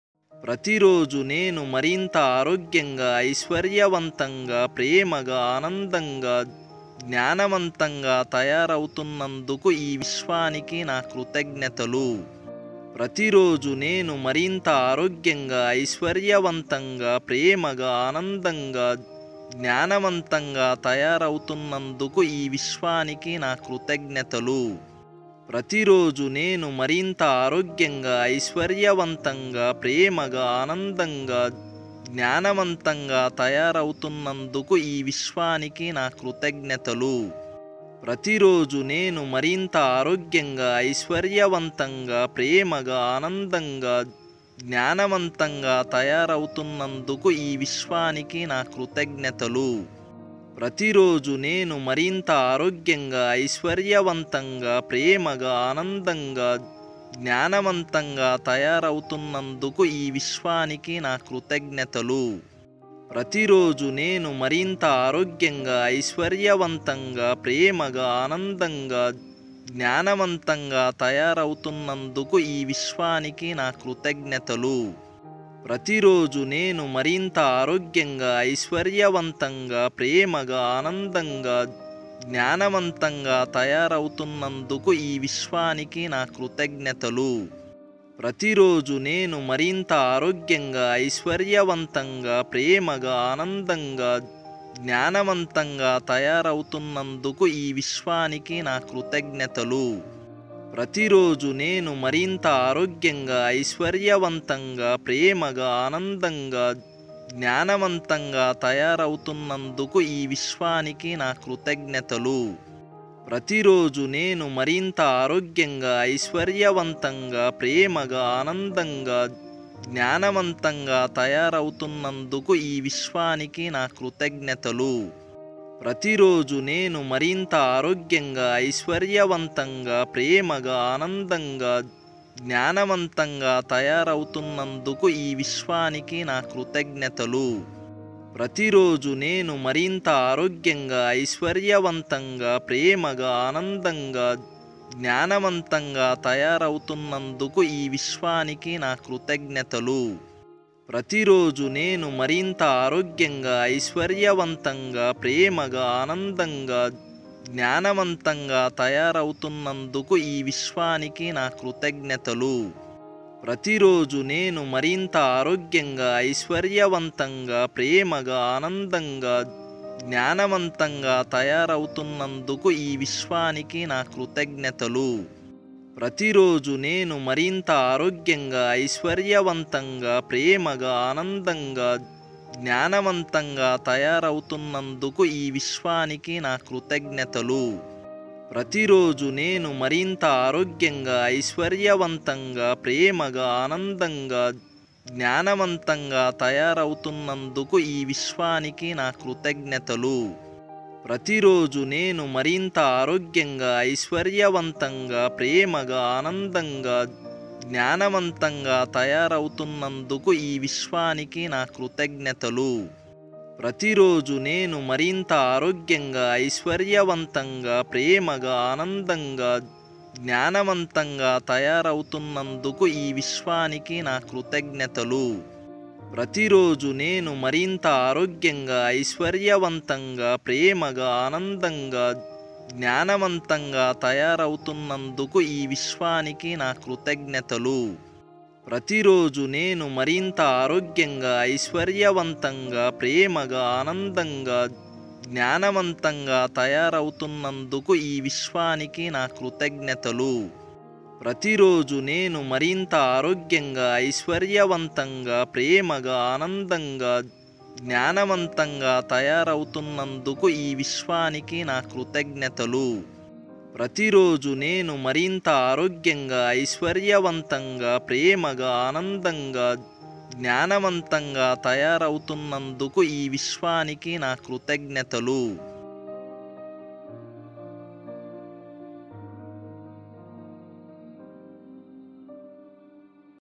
Positive affirmations / meditation in Telugu 5
Please listen this positive affirmations  day to day   continues 21 days or 41days and see the miracles which will be happen in your life . if you believe and listen then will be happen so listen continuously  all the best  today start your new journey.